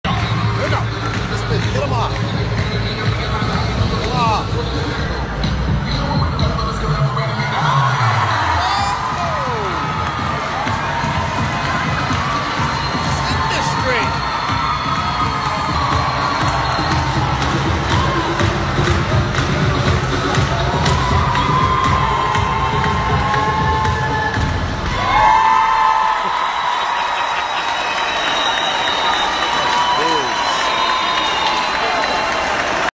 Police dance with flash mob at mall
FLASH MOB COPS: At first, police looked like they were going to try and break up a flash mob at a South Florida mall – but instead they got down and put on a show for Christmas shoppers.